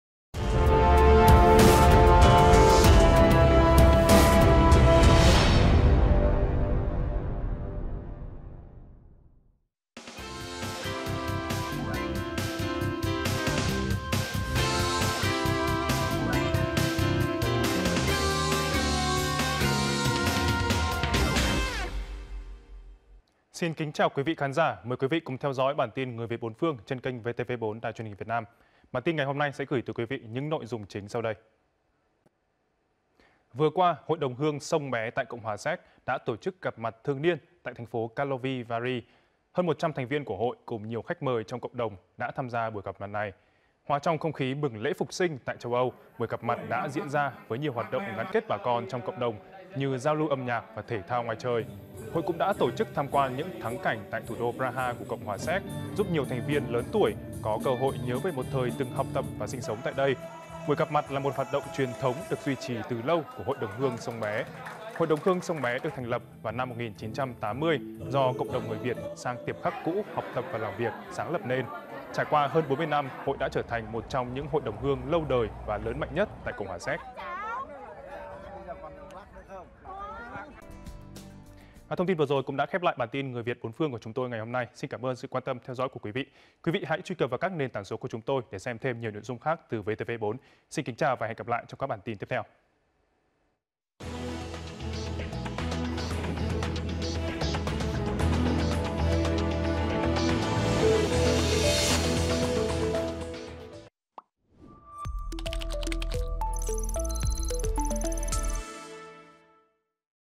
VTV4 | HĐH SÔNG BÉ TẠI CHÂU ÂU HỌP MẶT THƯỜNG NIÊN 2023 TẠI NOVÁ ROLE - CH SÉC | JŨNG STEPHAN
Phan-Nguyễn Studio
Thực hiện: Cộng tác viên Jũng Stephan - Minh Tâm tại CHLB Đức